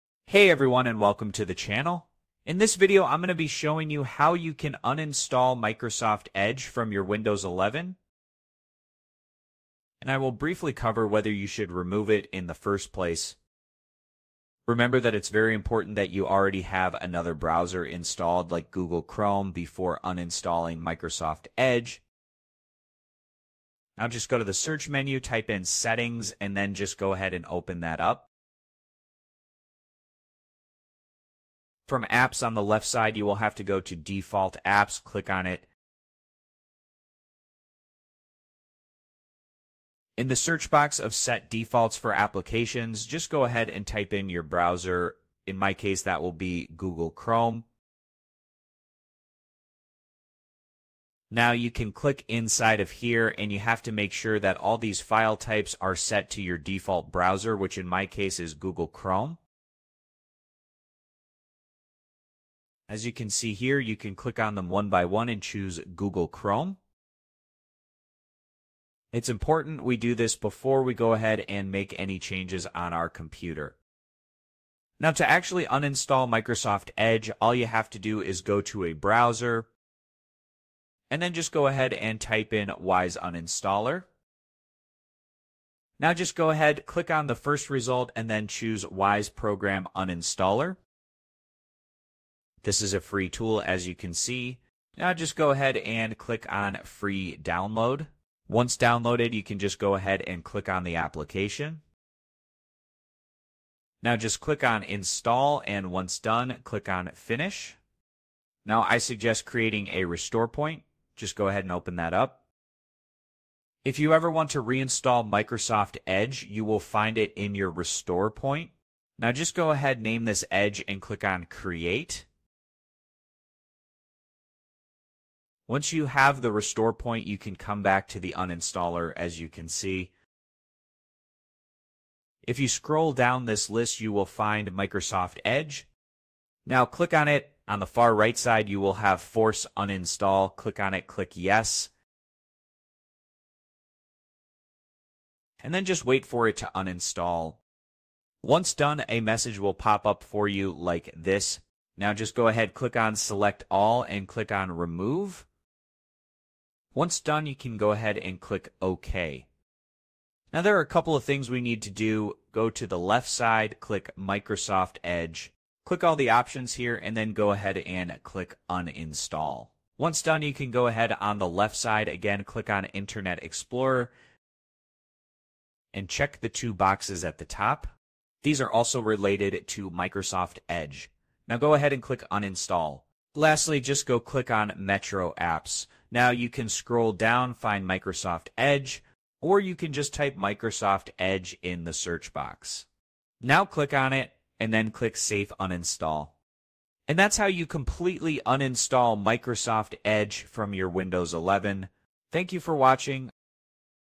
How to Uninstall Microsoft Edge in Windows 11/10 – Step-by-Step Guide 2025
Are you tired of Microsoft Edge being the default browser on your Windows PC? Whether you're looking to free up space, switch to another browser, or simply prefer not to have Edge on your system, this tutorial will show you how to completely uninstall Microsoft Edge from Windows 11 and Windows 10 with easy-to-follow steps.

🔹 What You’ll Learn in This Video:
✅ How to check if Microsoft Edge is removable on your PC
✅ Step-by-step guide to uninstall Edge via Settings
✅ How to use Command Prompt (CMD) to force uninstall Microsoft Edge
✅ How to remove Edge updates to prevent it from reinstalling
✅ Extra tips to set a different default browser after uninstalling Edge

📌 Timestamps
⏱ 00:00 - Introduction
⏱ 00:39 - Setting defaults for applications for your browser
⏱ 01:20 - Searching for Wise Uninstaller
⏱ 01:34 - Downloading Wise Uninstaller
⏱ 01:48 - Creating a restore point
⏱ 02:16 - Force uninstall Microsoft Edge

Microsoft Edge is deeply integrated into Windows, making it difficult to remove. However, with the right steps, you can completely uninstall it and take full control over your browser choices. This guide is simple, effective, and suitable for beginners and advanced users alike!

🔥 Found This Video Helpful? Support Us!
✅ Like 👍 the video to help others find it
✅ Share 📢 this tutorial with friends who need it